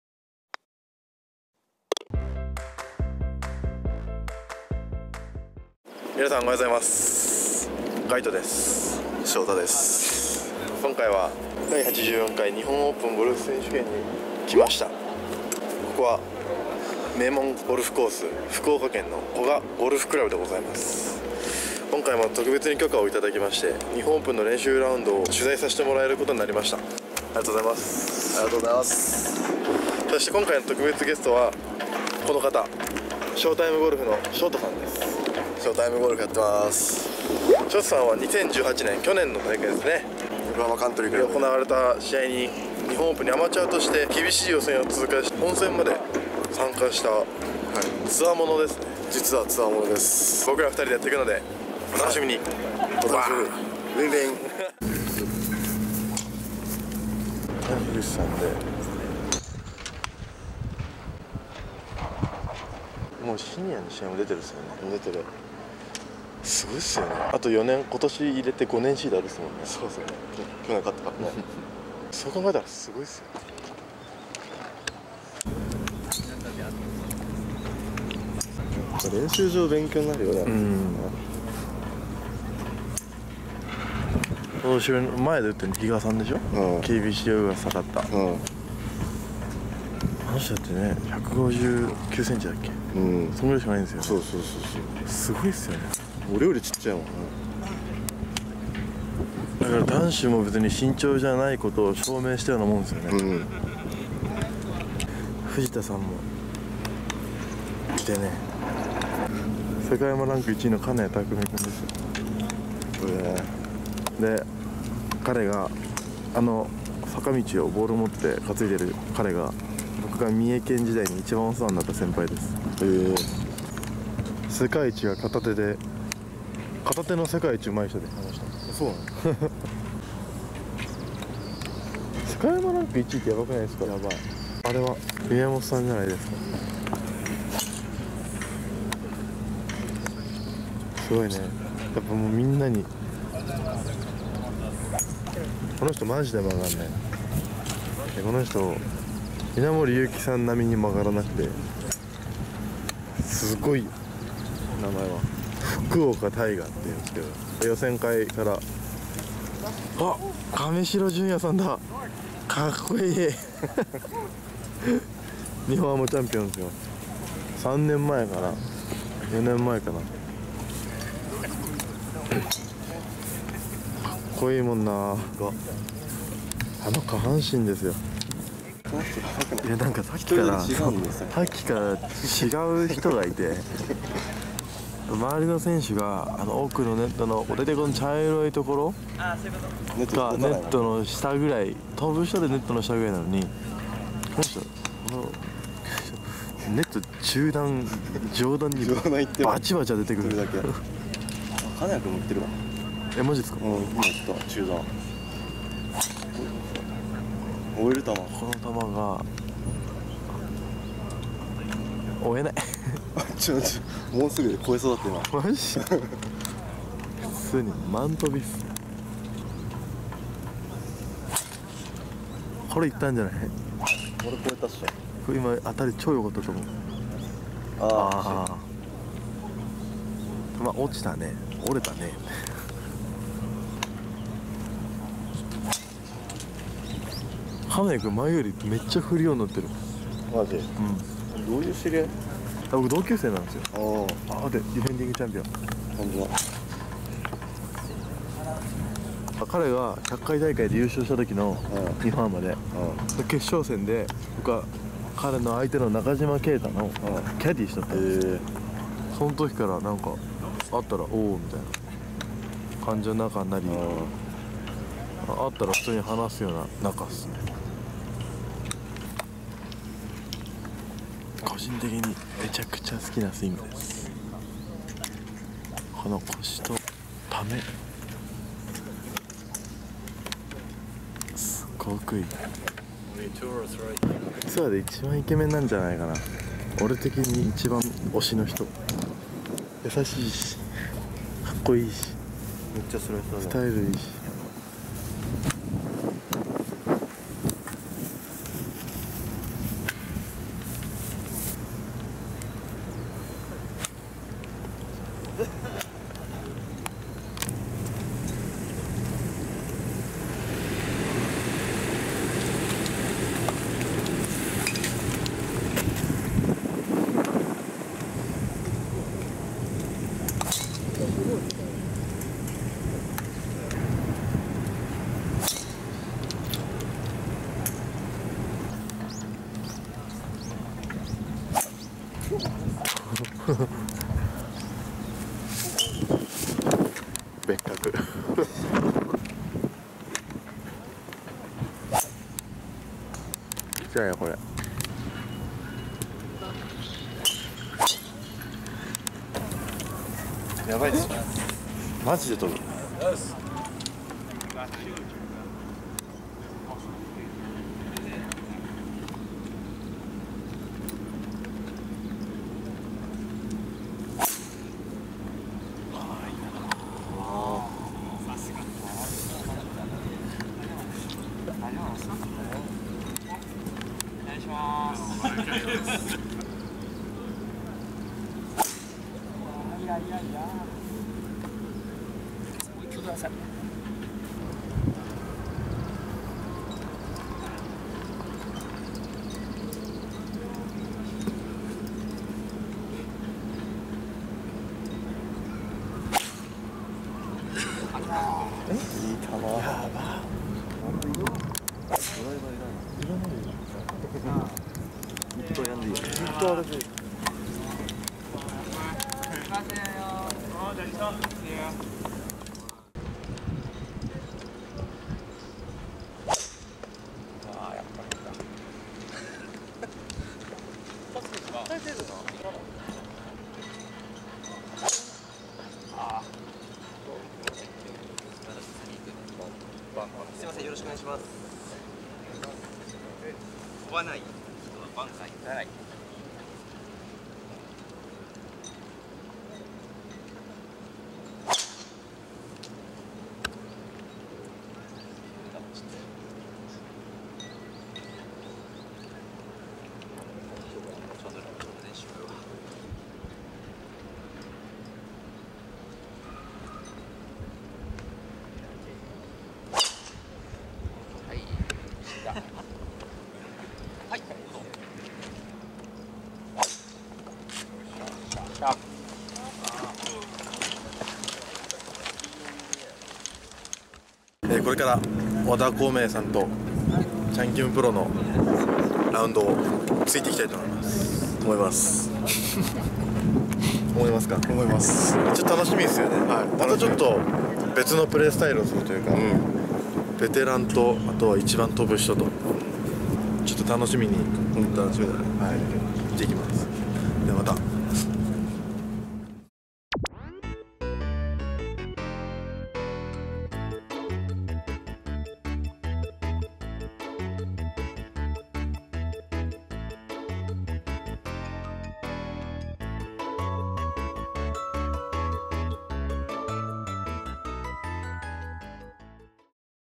【興奮！】日本一のゴルファーを決める【日本オープンゴルフ選手権】にやってきた！
本日は、特別な許可をいただき、古賀ゴルフ・クラブで行われている
日本オープンの練習ラウンドを取材させてもらえることになりました！！
カイトくんとShotaさんの2人で取材します。
Shotaさんは2018年、日本オープンにアマチュアとして、厳しい予選を通過し、みごと本選まで参加されています。
まずは、練習場の模様と1番ホールのティーショットです。

NHKの放送情報はこちら！

♣ringlf グッズ・ウェアについてはこちらから！

♣ringolfオープン2019開催スケジュールはこちら！

おすすめ再生リスト ーーーーー

いろいろ ーーーーー